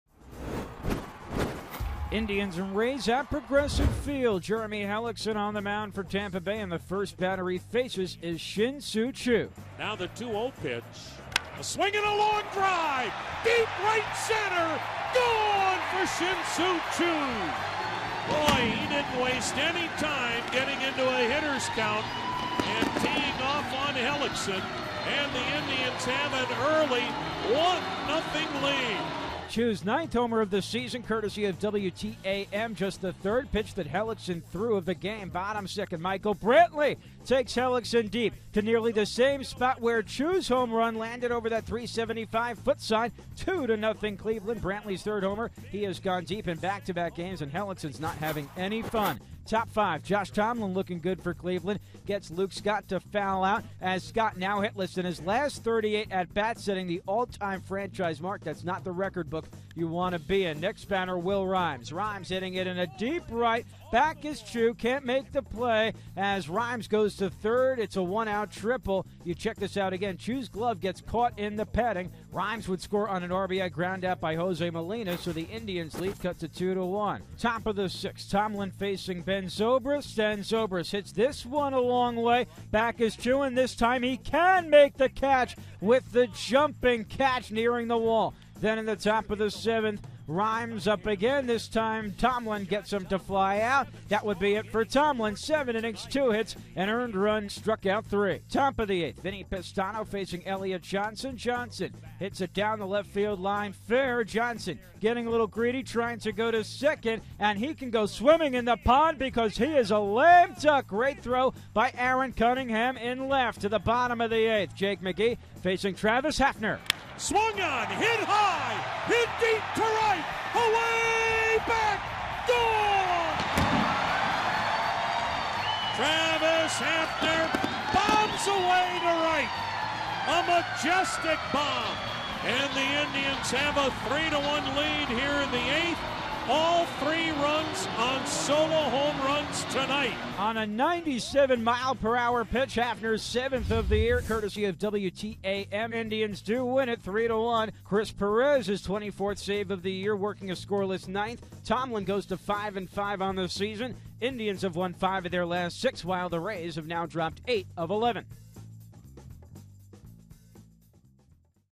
2012/07/05 Recap: TB 1, CLE 3
7/5/12: Backed by solo blasts, Tomlin shuts down Rays